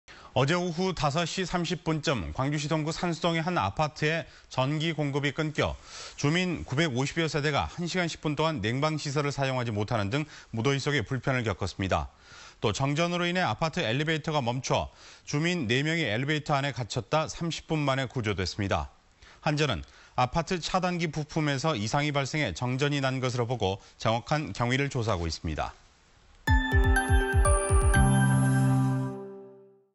광주 한 아파트서 정전..950여세대 불편
어제(8) 오후 5시 30분쯤,
광주시 동구 산수동의 한 아파트에
전기 공급이 끊겨 주민 950여세대가
1시간 10분 동안 냉방시설을
사용하지 못하는 등
무더위 속에 불편을 겪었습니다.

또 정전으로 인해 아파트 엘리베이터가 멈춰서
주민 4명이 엘리베이터 안에 갇혔다
30분만에 구조됐습니다.

한전은 아파트 차단기 부품에서
이상이 발생해 정전이 난 것으로 보고
정확한 경위를 조사하고 있습니다.